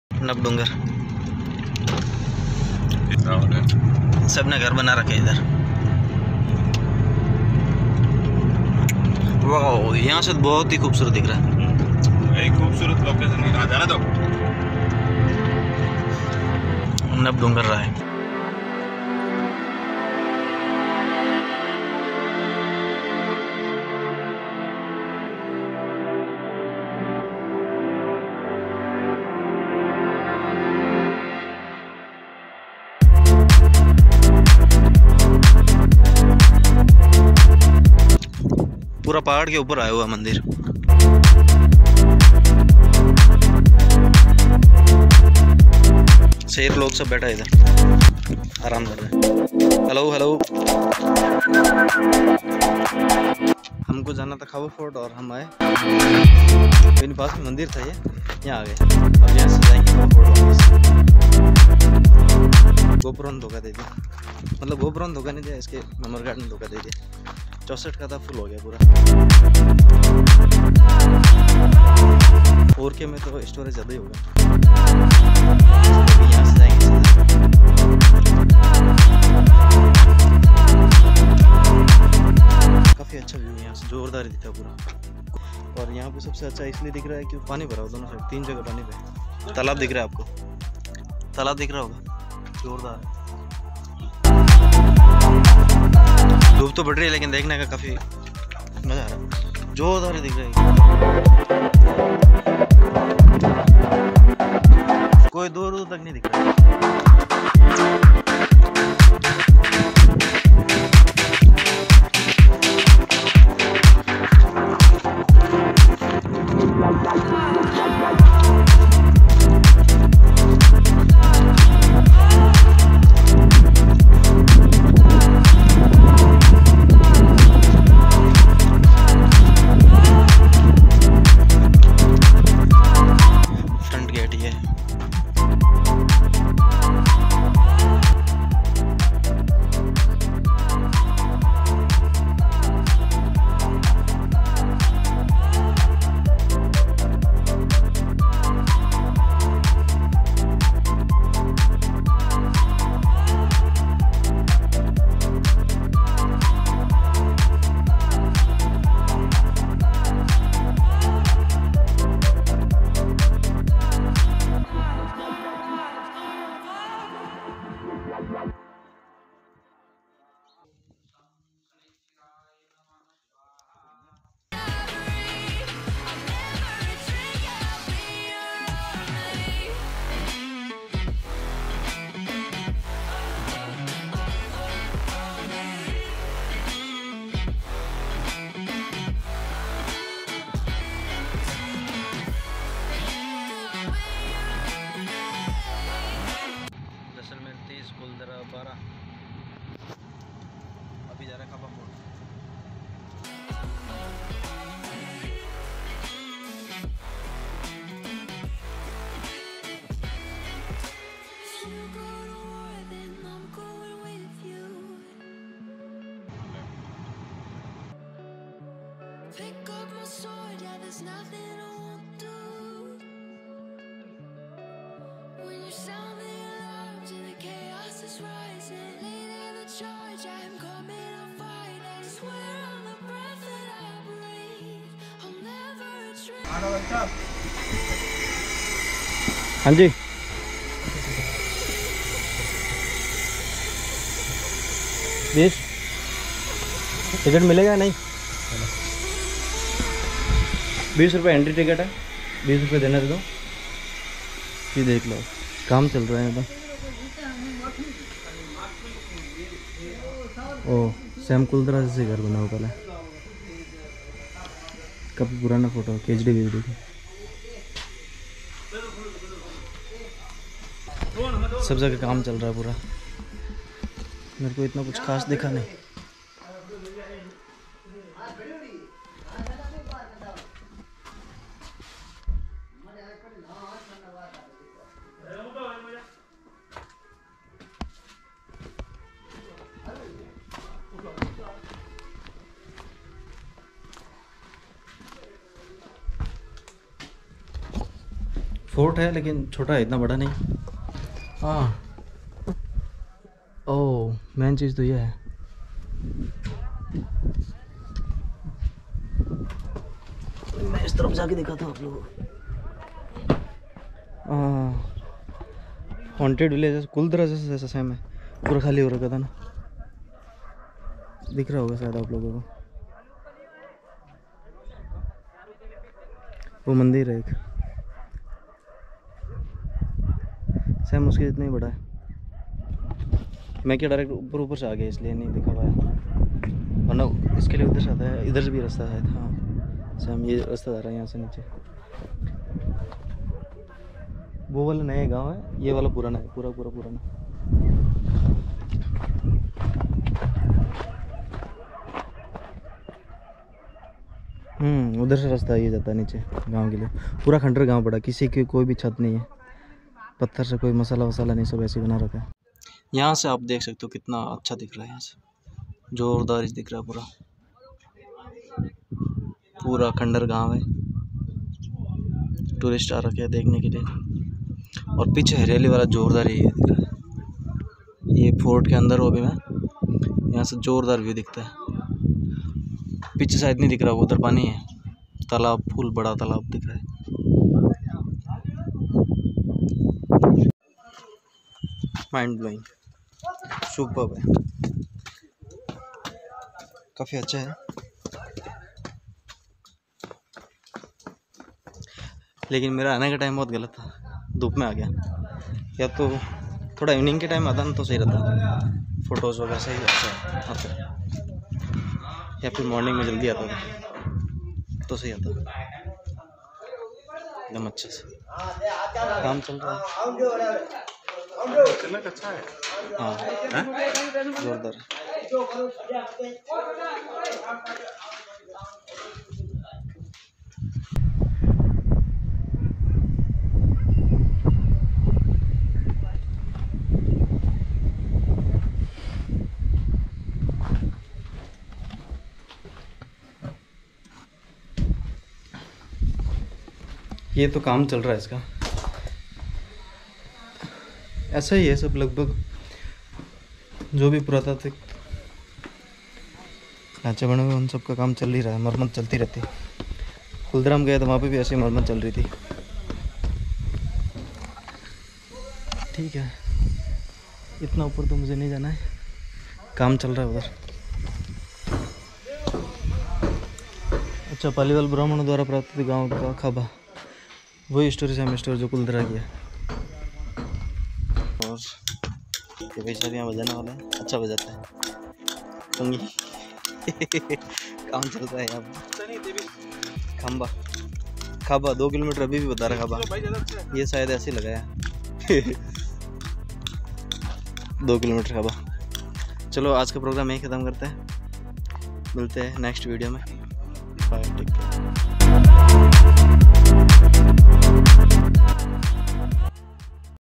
Khaba Fort Jaisalmer / Nabh Dungar / Mr. Deora
Khaba Fort Jaisalmer

The beautiful state of Rajasthan with its rich cultural and architectural masterpieces is famous worldwide for its magnanimous fort, grand palaces and fortresses. Shrouded in mystery and awe, the Khaba Fort is yet another enchanting fort in Jaisalmer. Once a native place of the Paliwal Brahmins, the fort is said to be deserted overnight under mysterious circumstances. Visitors can take the chance to look at the ruins that once served as palatial homes for 80 families for over two centuries ago. Take the opportunity to hear the mysterious story of this deserted fort from any of the nearby professional guides offering such service